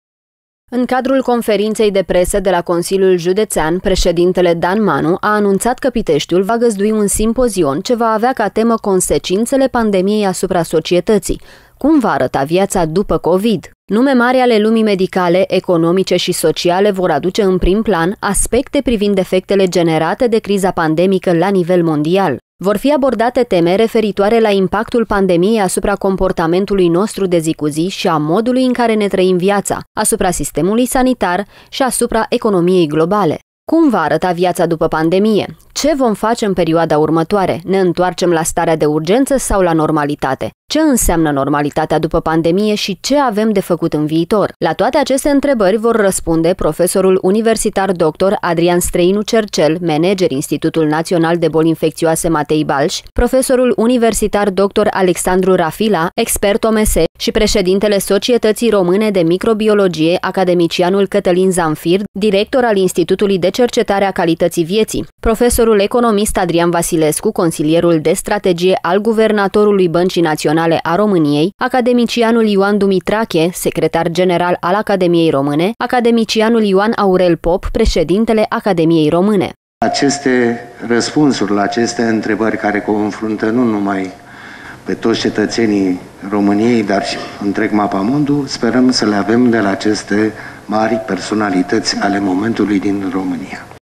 stire 05 06 2020 Viaţa după pandemie
Joi, 11 iunie, la ora 10.00, Consiliul Judeţean Argeş, în parteneriat cu Academia Română, organizează Simpozionul cu tema „Consecinţele pandemiei asupra societăţii. Cum va arăta viaţa după COVID?”. Vor conferenţia nume mari ale medicinei române, printre care profesorul universitar doctor Adrian Streinu Cercel şi profesorul universitar doctor Alexandru Rafila.